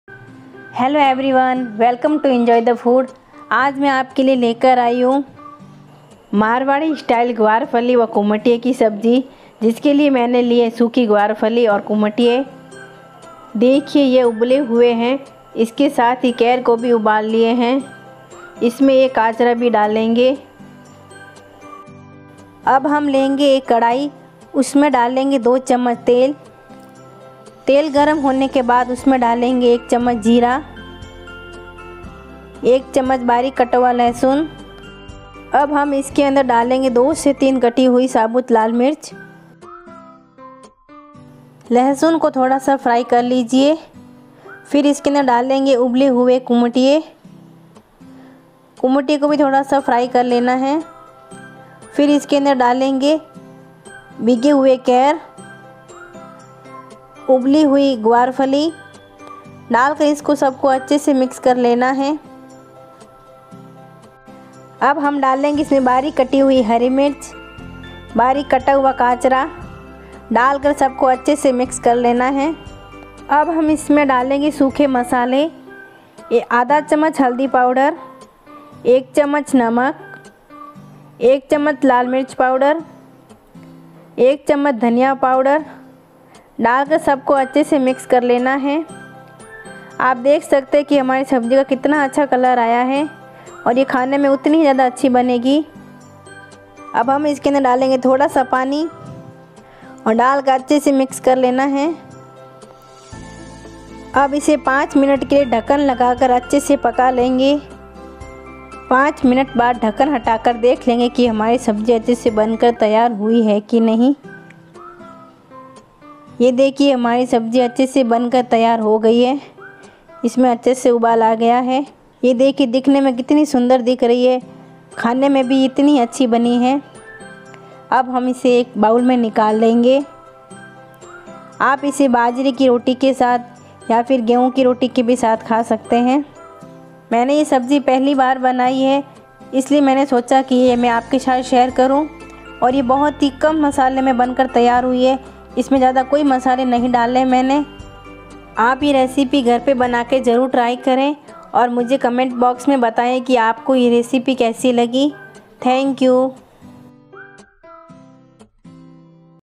मारवाड़ी सब्जी | केर गवारफली की देसी स्टाइल सब्जी | Marwadi style saabji by Enjoy The Food
राजस्थानी केर कुठिया सब्जी

ग्वारफली -1 कटोरी
केर - 1 कटोरी
कुमठिया - 1 कटोरी

लहसुन - 1 चम्मच
लाल मिर्च
केर
ग्वार फली
हरी मिर्च
हरी धनिया
कुमथिये
हल्दी पाउडर
नमकी

Rajasthani Ker Kumthia Sabzi

Guarfali - 1 bowl
Ker - 1 bowl
Kumthia - 1 bowl

Red chilly
ker
Guar pods
Green chilli
green coriander
wither
turmeric powder
salt